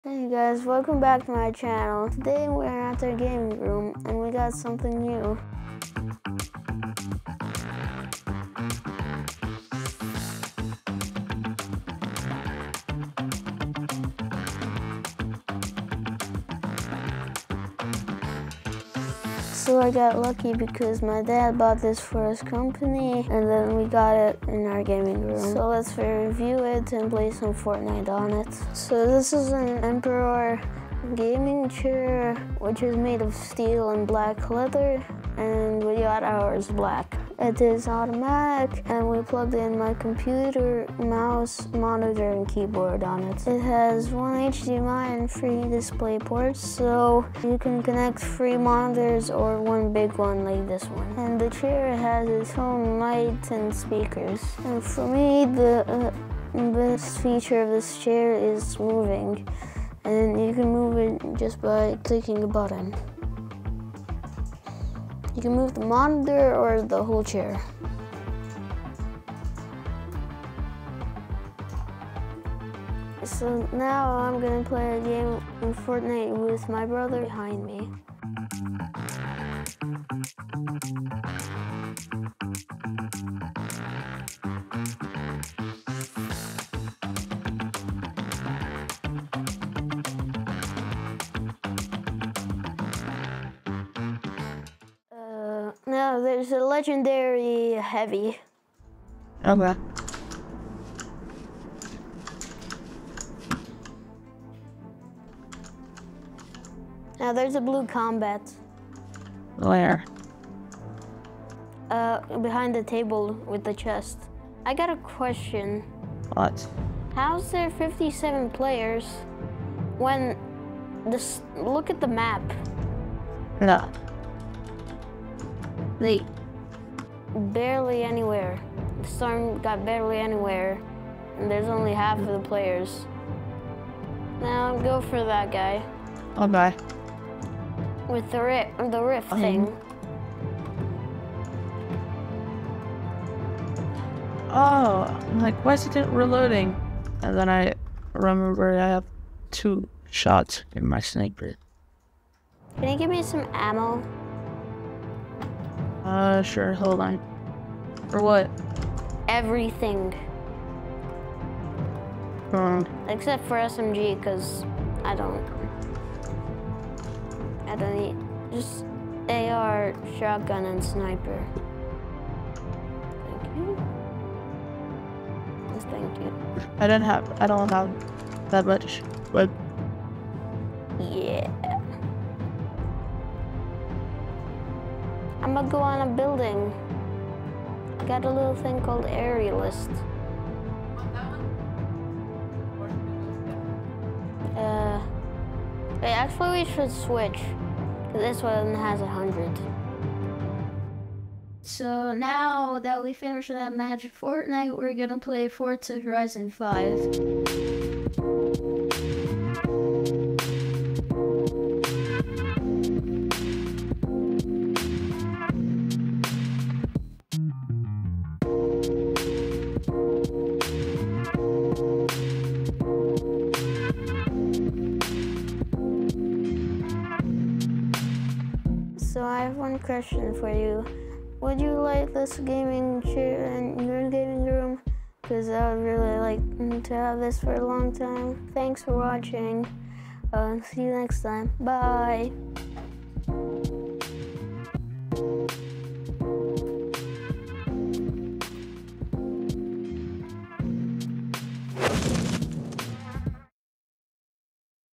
Emperor Gaming Chair - Testing and Playing
In today's video we have a really cool and big gadget, it is Emperor gaming chair... Playing games in it is a whole new level of playing...I recomend you to check this video and see do you also like it...